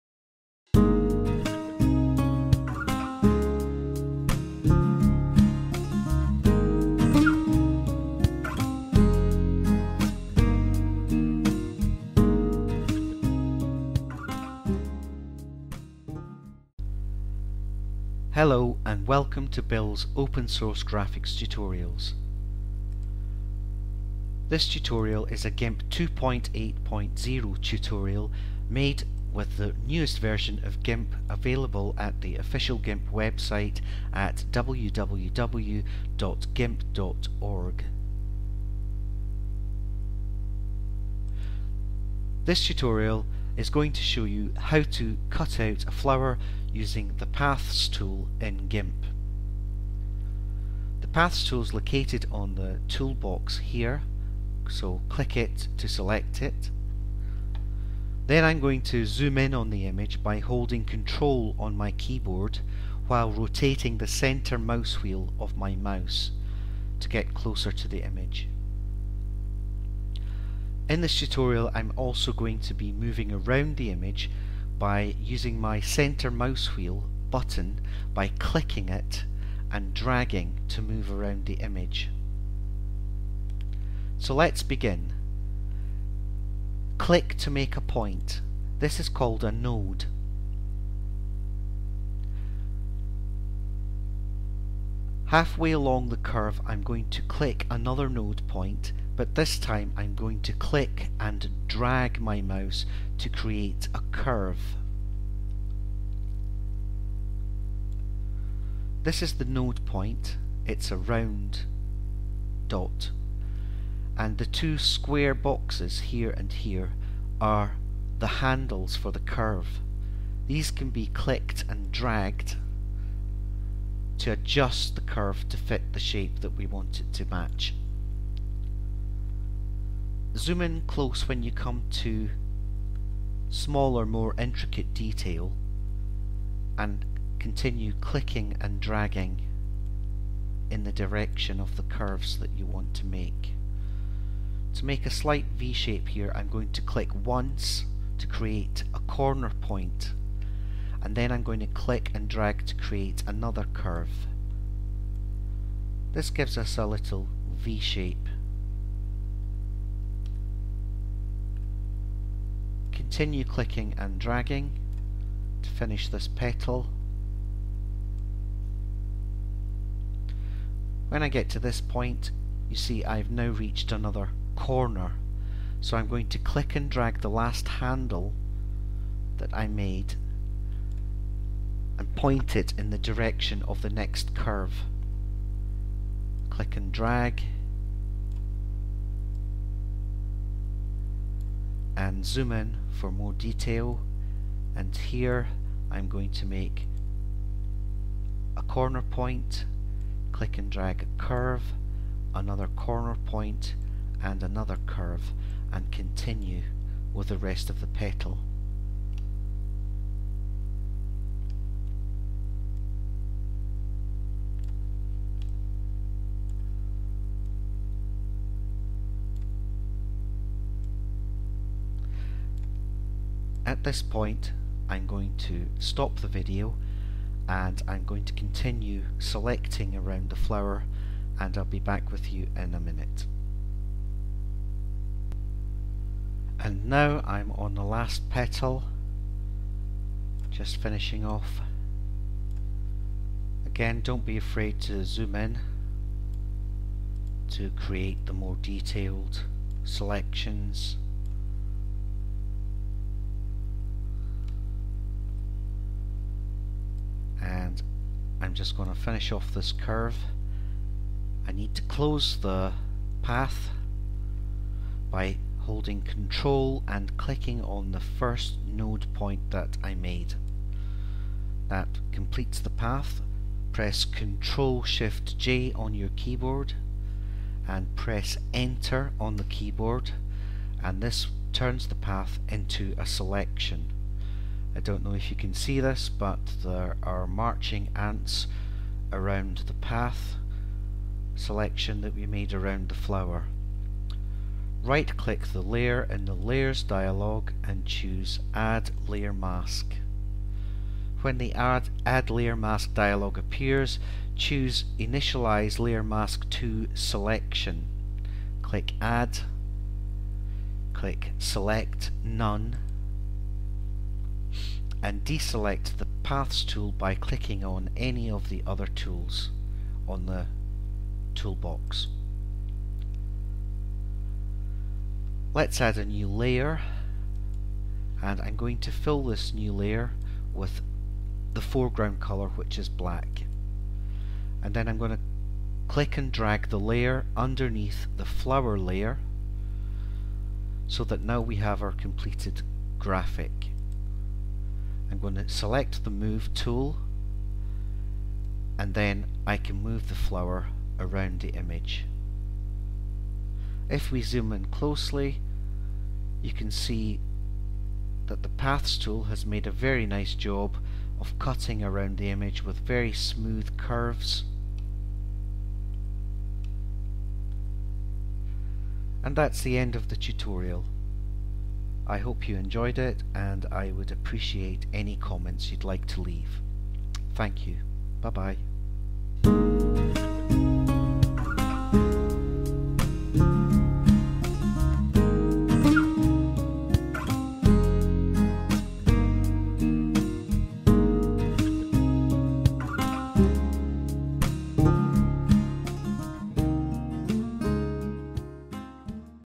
GIMP Tutorial - Using the paths tool to cut around an image
This is the first in a new series of GIMP tutorials using the newest version, GIMP 2.8.  Also a first for me, it's a tutorial with a voice over!!!  For those interested in my accent, I'm Scottish.  This tutorial is in HD so if you want to see it better, then view in full screen in HD.